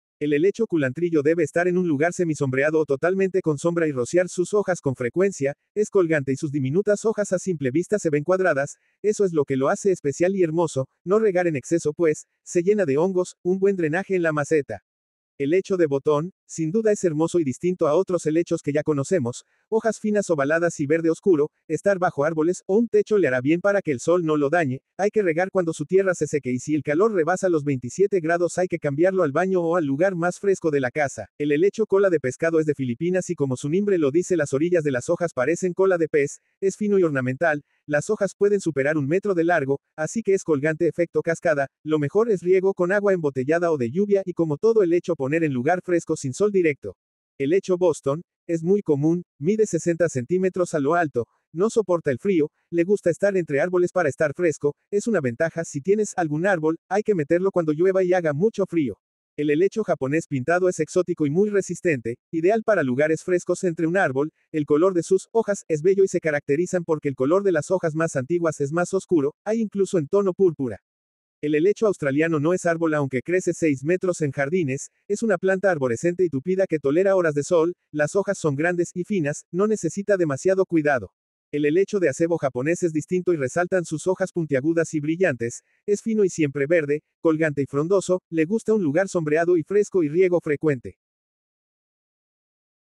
7 HELECHOS bonitos para EXTERIOR
Varios tipos de Helechos que necesitan la frescura exterior para crecer sanos y de fácil mantenimiento